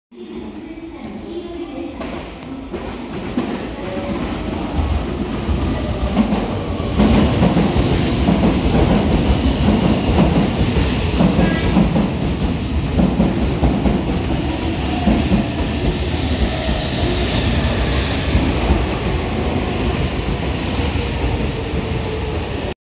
●20220601 2200系 到着 ＠金山
撮影路線：名鉄 名古屋本線
撮影駅：金山
行先：特急 新鵜沼
車両：2200系
両数：6両
撮影機材：【スマホ】SONY Xperia XZ2 Premium
MH：なし
備考欄：